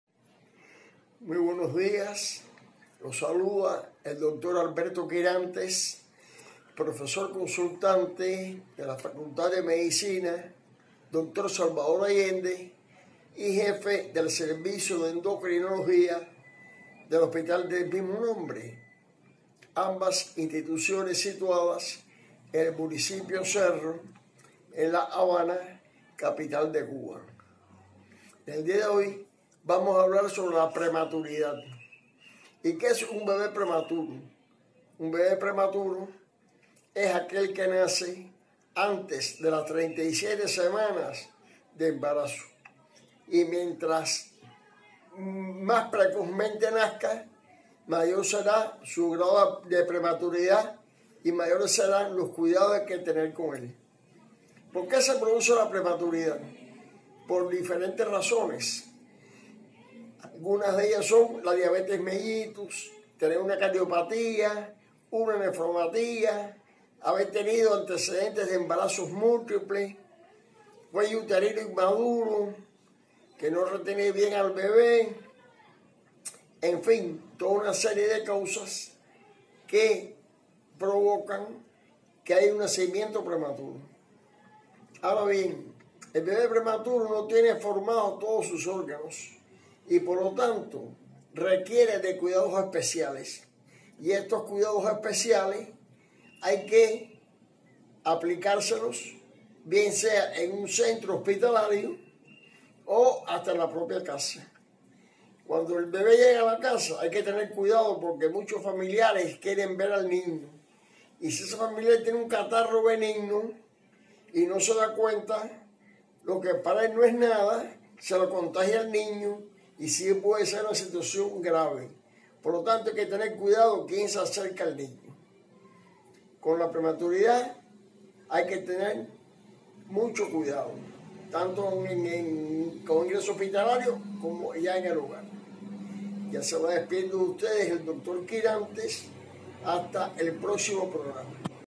consultasmedicas | El bebé prematuro y sus cuidados
Un bebé prematuro es aquel nacido antes de las 37 semanas completas de gestación, o sea, más de 3 semanas previas a la fecha pronosticada del parto. Si una mujer comienza su trabajo de parto antes de las 37 semanas, se le llama trabajo de parto prematuro. La prematuridad fue un motivo notable de fallecimientos infantiles pero el progreso en las tecnologías médicas y del apoyo a recién nacidos ha incrementado la supervivencia en los bebés prematuros.

Acá te contamos más detalles...